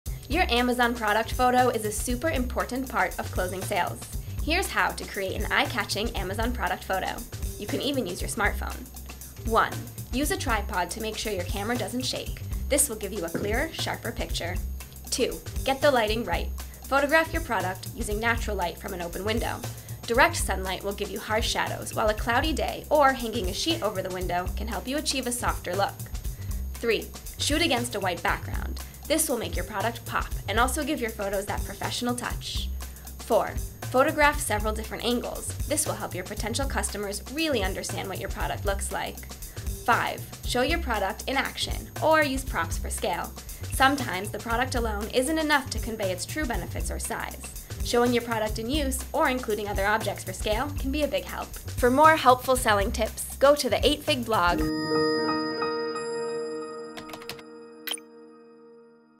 8fig Tiny Tips: Create Eye-Catching Product Photos
Your product photos are a super important part of your product listing. Here’s how to make sure they catch your customers’ eye and help you sell. You can even use a tripod!

1. Use a tripod for a clear picture.
2. Get the lighting right.
3. Shoot against a white background.
4. Photograph several different angles.
5. Show your product in action.